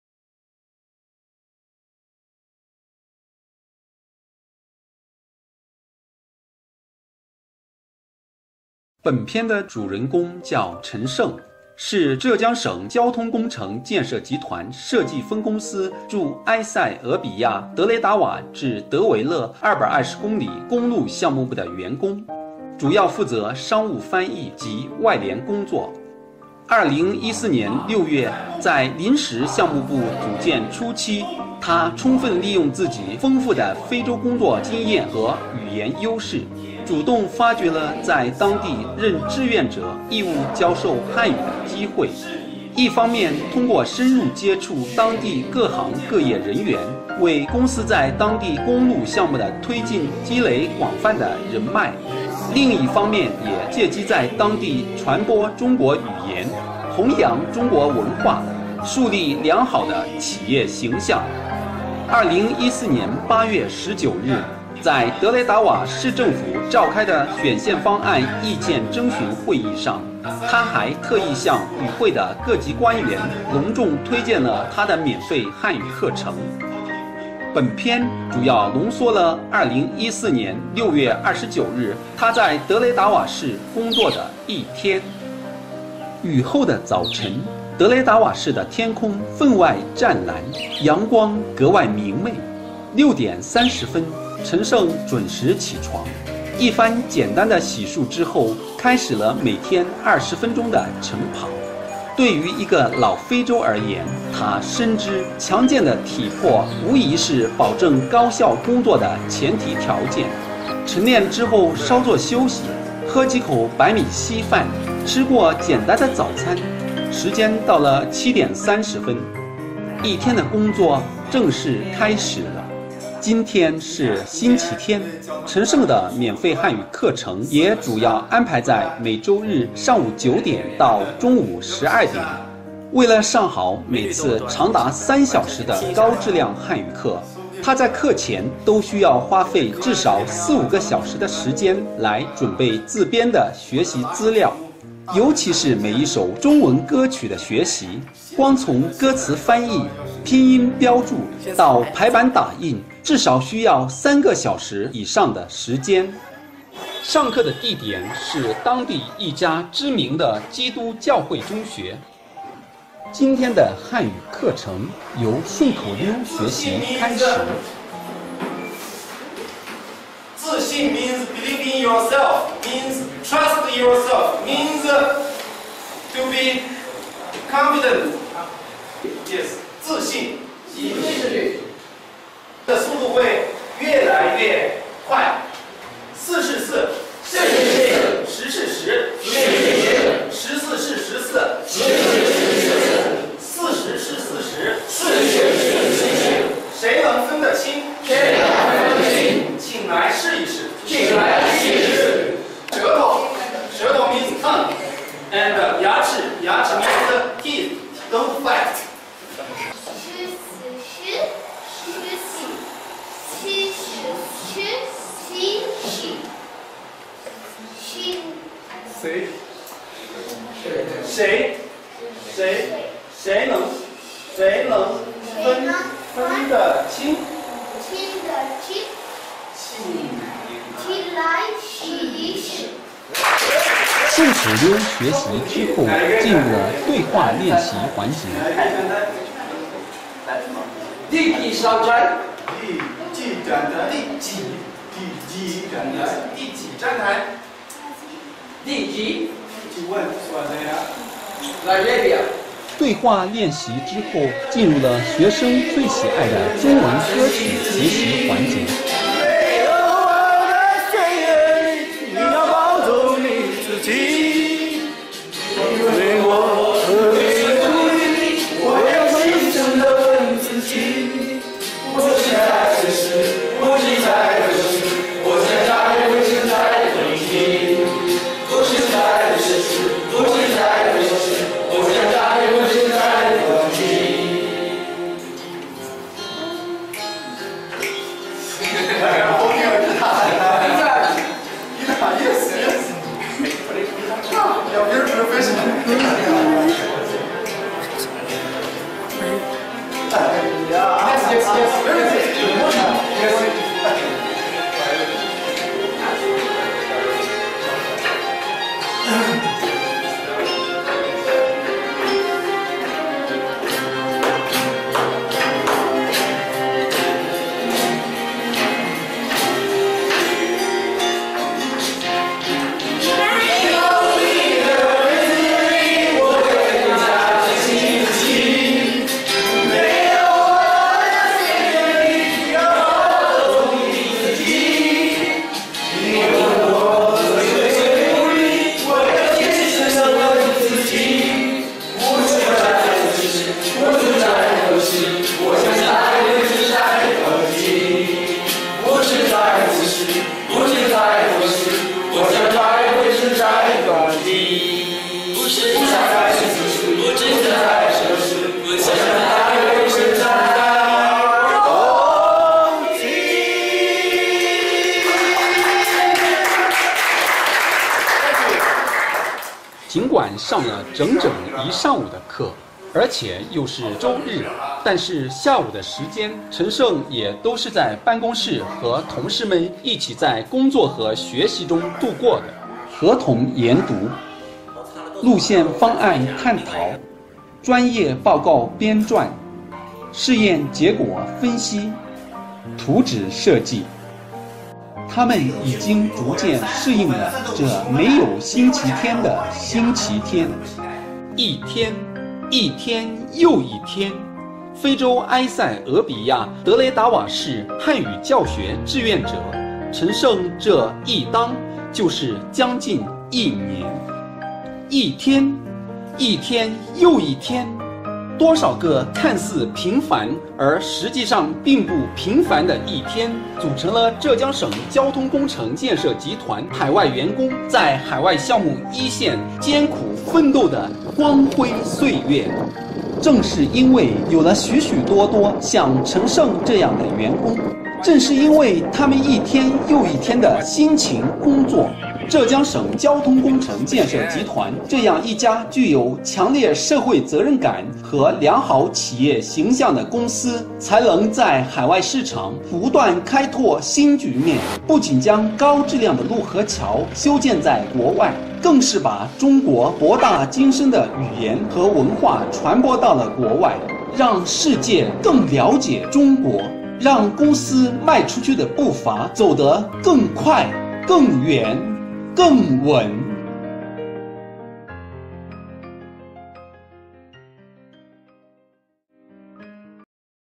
One Day in Dire Dawa, Ethiopia as A Voluntary Chinese Language Teacher
This movie mainly concentrates one day of Mr. Chen Sheng's Chinese language teaching in Dire Dawa, Ethiopia, as a volunteer teacher. It is just a very good memory of his unforgettable experience in Ethiopia, Africa.